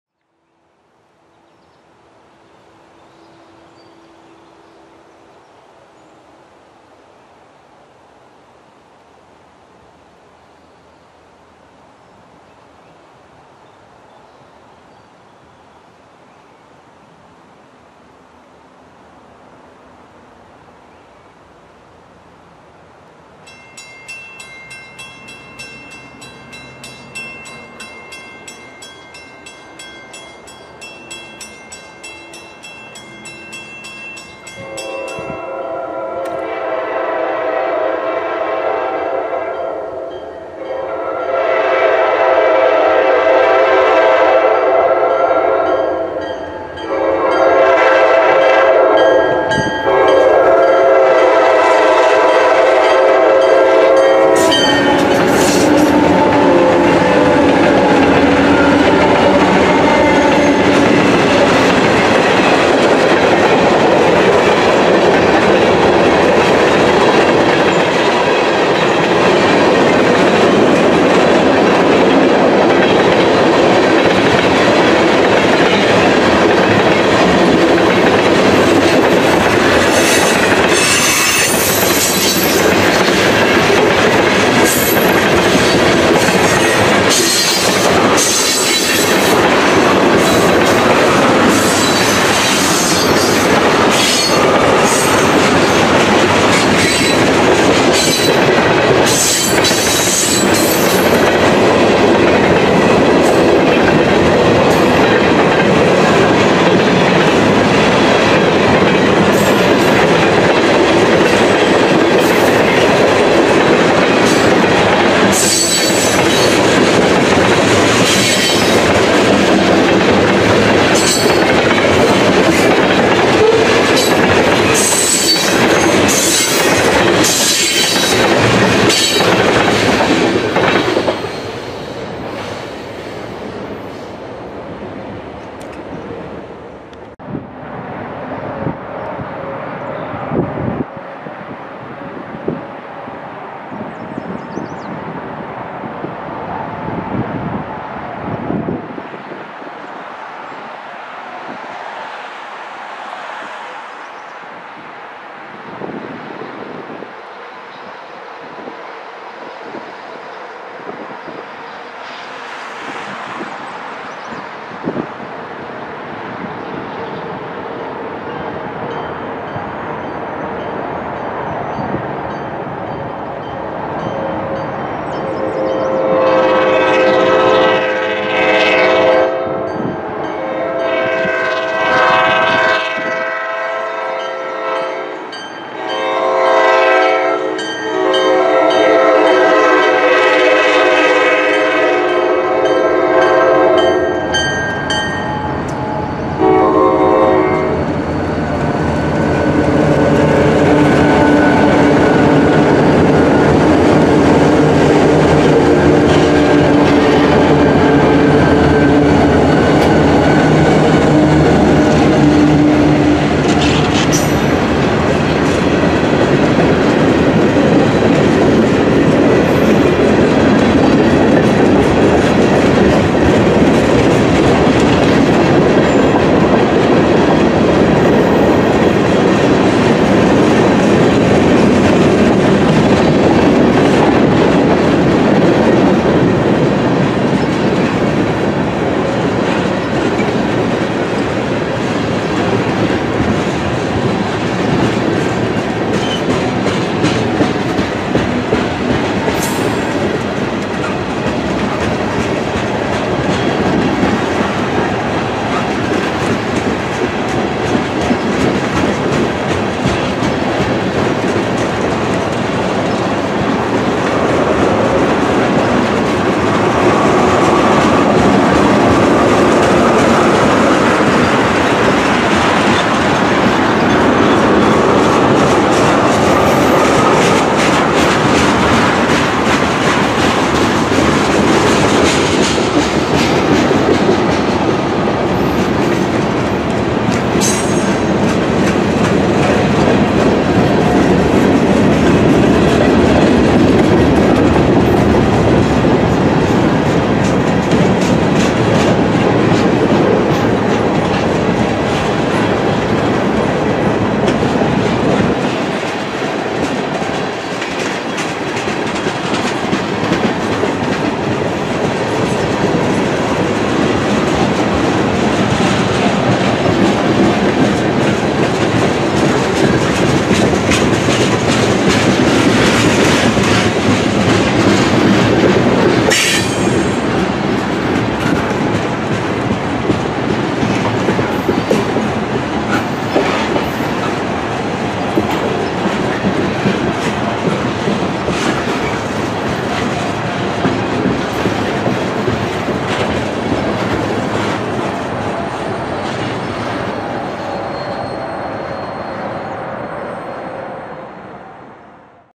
A Rare Daytime Chase of Buffalo & Pittsburgh SIBF 5/12/21
Buffalo & Pittsburgh Manifest SIBF normally runs at Nighttime but due to track work in Salamanca, NY, it is running during the day for the 2nd week of May 2021. We first catch the Train in South Wales, NY and then we chase it North to West Seneca, NY. I hope you guys enjoy the video!